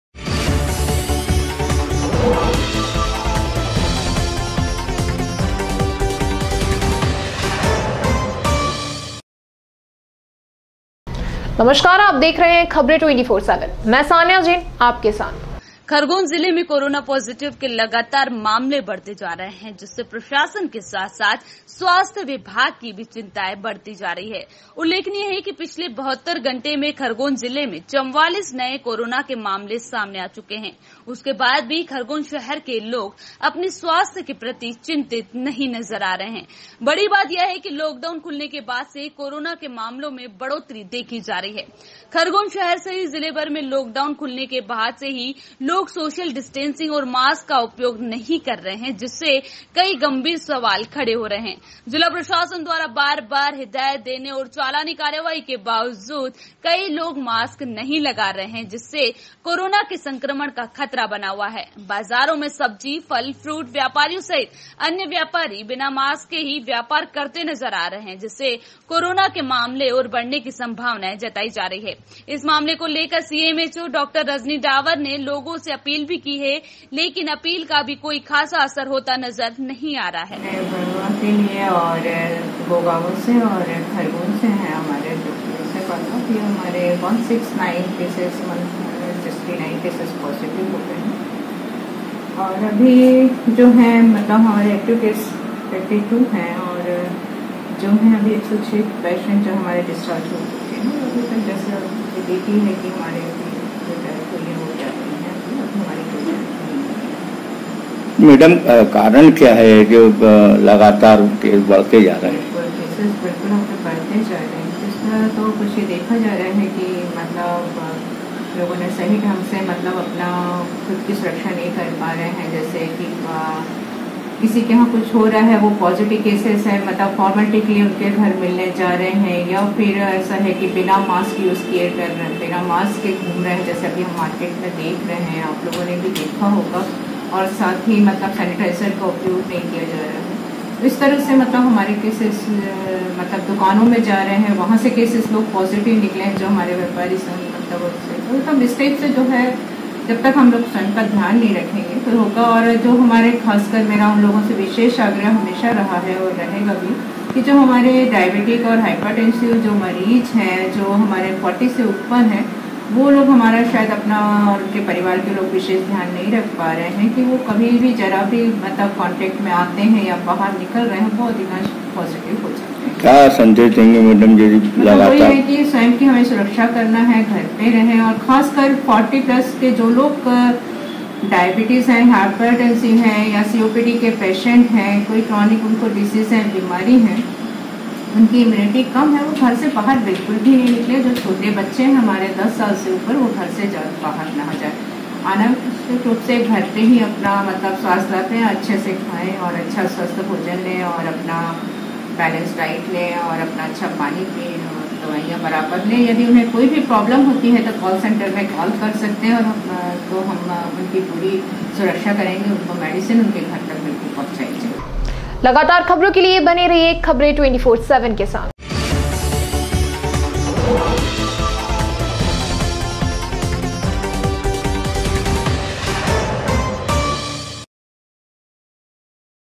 Khabren24x7 खरगोन जिले में कोरोना पॉजिटीव के लगातार मामले बढ़ते जा रहे है 03.06.2020
खरगोन जिले में  कोरोना पॉजिटीव के लगातार मामले बढ़ते जा रहे है